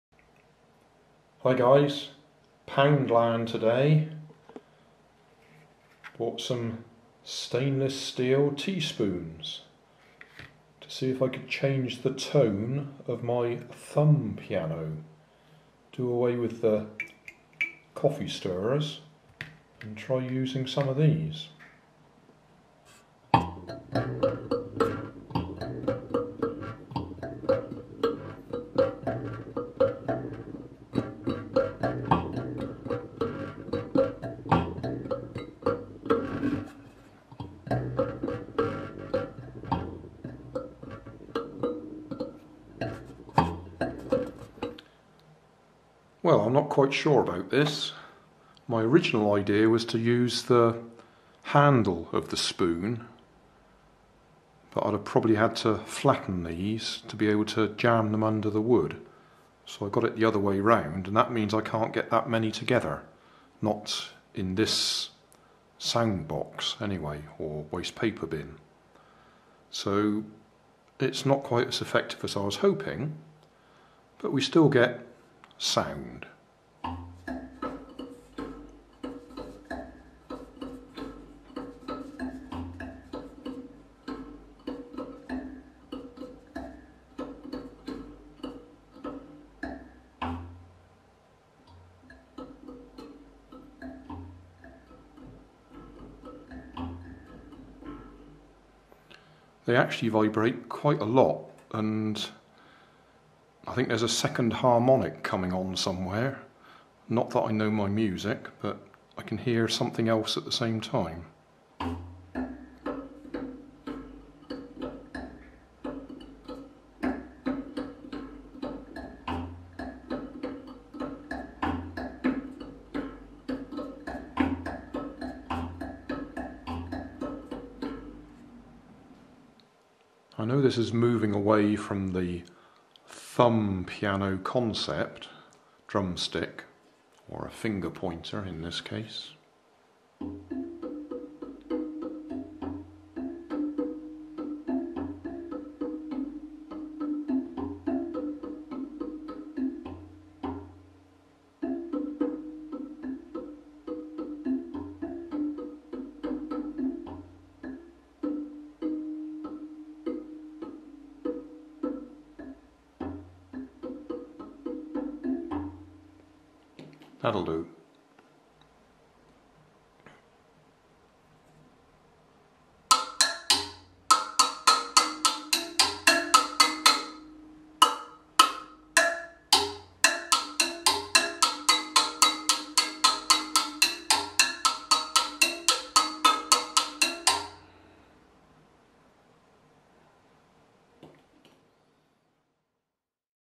Thumb Piano Tea Spoon version
Thumb Piano Tea Spoon version. This is my second version of a thumb piano. The first version is here using coffee stirrers
kirti singh asked "can you make a thumb piano?" so I did a YouTube search and found a couple of very simple basic thumb pianos. I will put the links to those videos below in my video description so you can watch them for the build details.
I used scrap wood, coffee stirrers, coach bolts, wing nuts, Aluminium waste paper bin.
I am sure you can use many different components and still get something vaguely musical out of it.

This musical instrument is known by many names. Here is the Wikipedia article
teapot100 had this information in their video description - Scroll down to instructions found